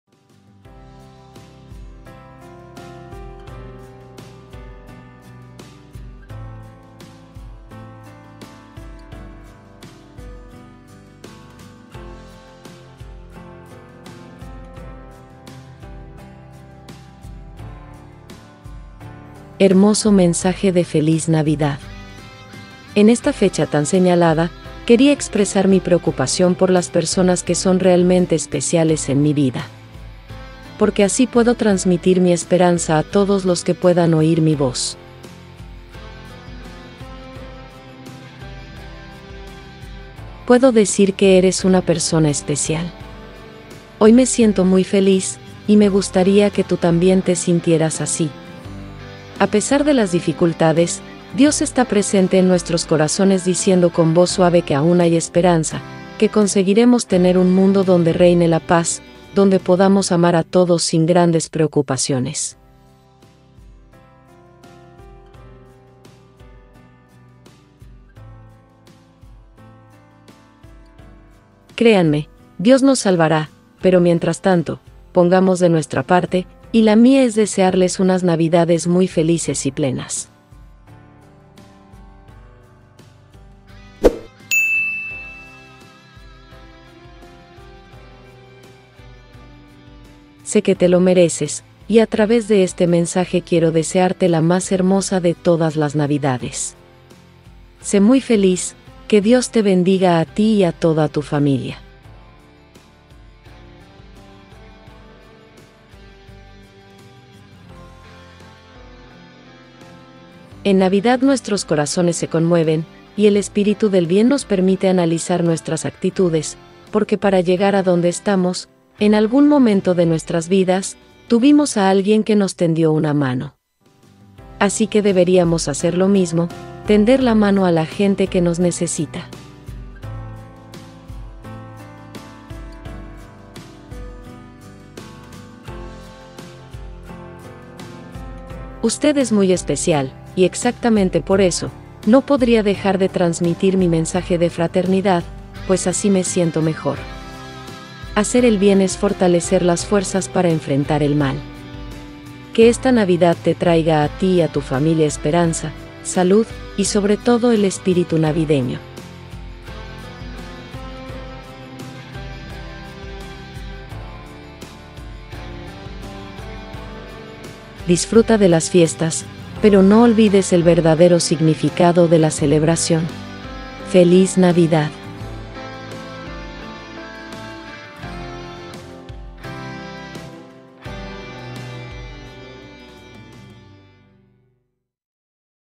¡Feliz Navidad 2022! [para ti] BONITA Mensaje de Feliz navidad PARA UNA PERSONA ESPECIAL
¡Feliz Navidad 2022! Que pases una feliz Navidad con mucho amor y felicidad. Felices fiestas.
Mensaje de Navidad para una persona especial.

Feliz Navidad 2022 para ti

Bello video de navidad.

En Navidad nuestros corazones se conmueven, y el Espíritu del Bien nos permite analizar nuestras actitudes, porque para llegar a donde estamos, en algún momento de nuestras vidas, tuvimos a alguien que nos tendió una mano.

Así que deberíamos hacer lo mismo, tender la mano a la gente que nos necesita.